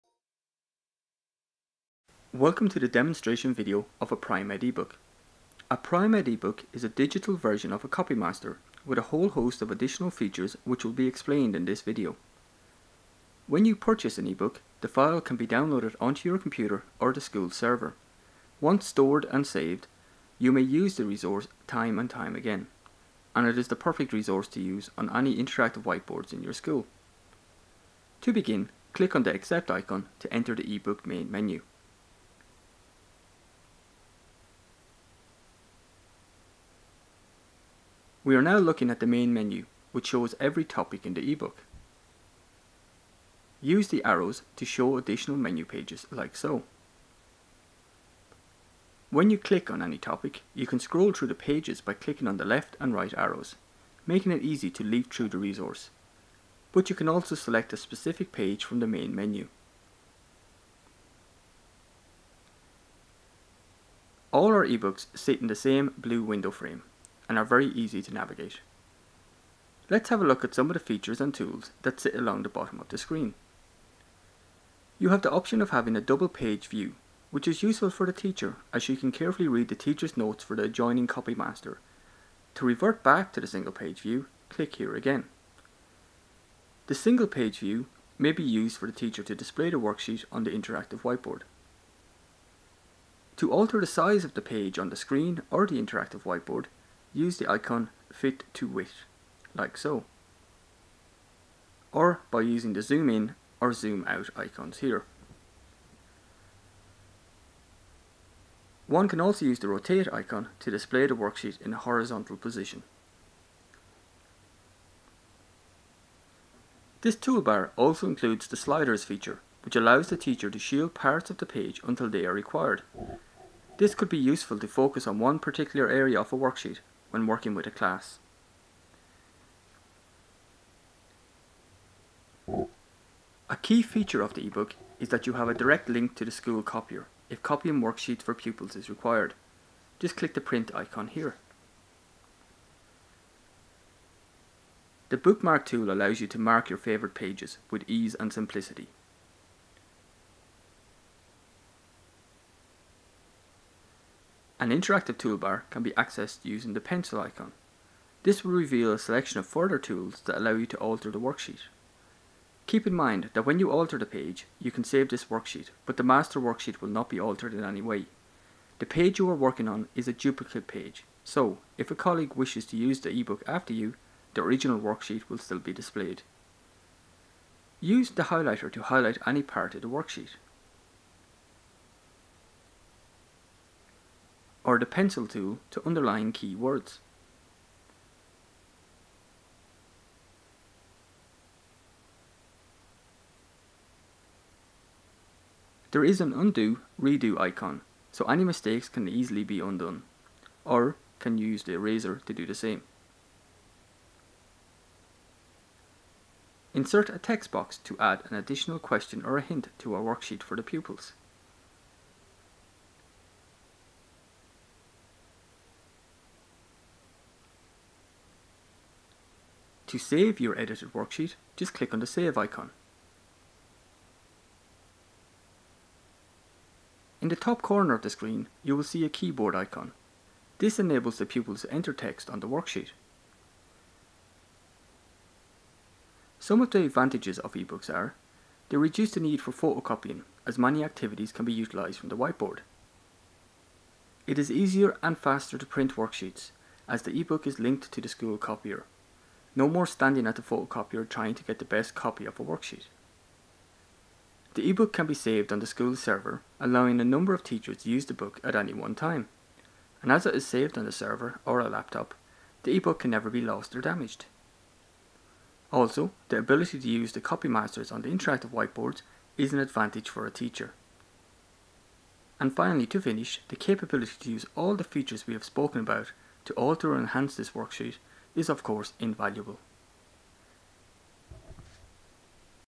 India eBook demo video
India - a cross curricular theme is a series of three books that present opportunities for pupils to discover some natural, physical, cultural, economic and political aspects about this fascinating and extremely diverse Asian country and its people. The books in this series give selected information about both modern and ancient India and use a wide variety of activities across many learning areas.